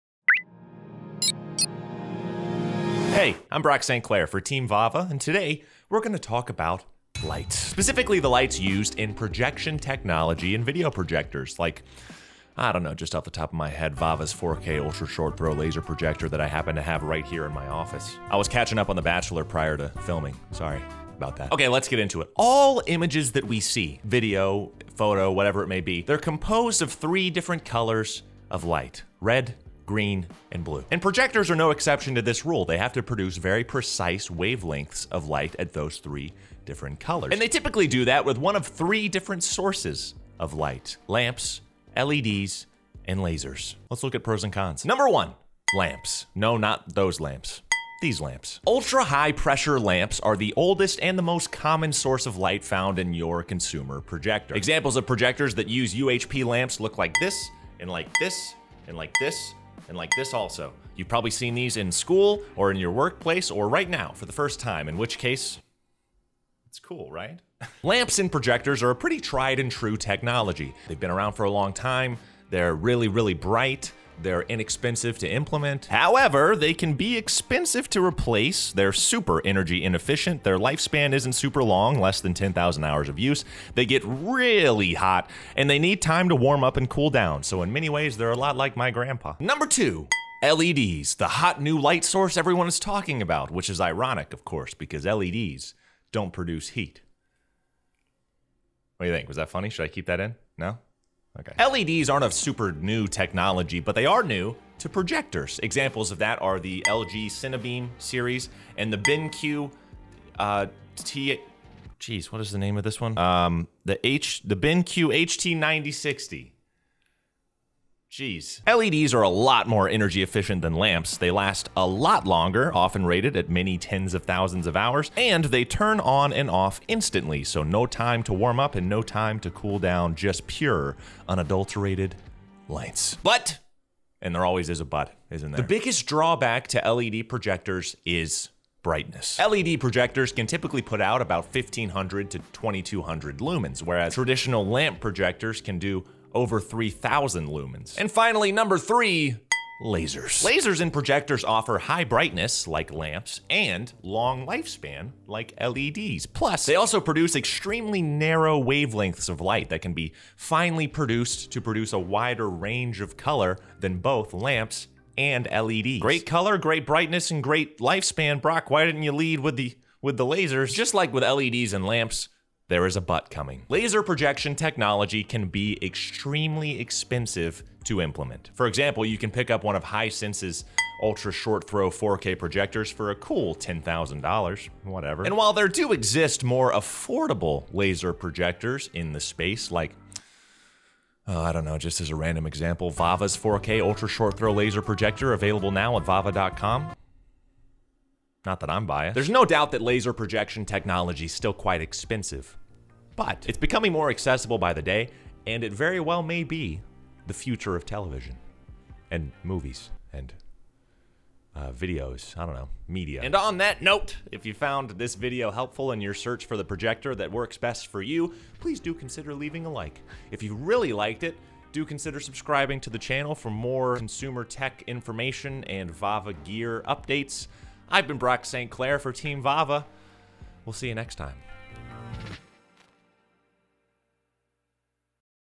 VAVA 4K Projector Laser Light Technology Comparison (Laser, LED, Lamps)
A quick overview highlighting the pro’s and con’s on Lamps, LED, and Laser light sources from tech enthusiast Brock Sinclair.

Our 4K projector’s superior image quality is due in part to the laser technology we use. In this video, we’ll explain the difference of how projectors using laser light offer all the best features found in current LED Projectors and traditional UHP Lamp projectors on the market. Laser tv’s maybe the future of home theatre, but at what cost? We discuss this further in our video.

What are your thoughts on how new Laser TV’s compare to other TV’s using traditional light sources?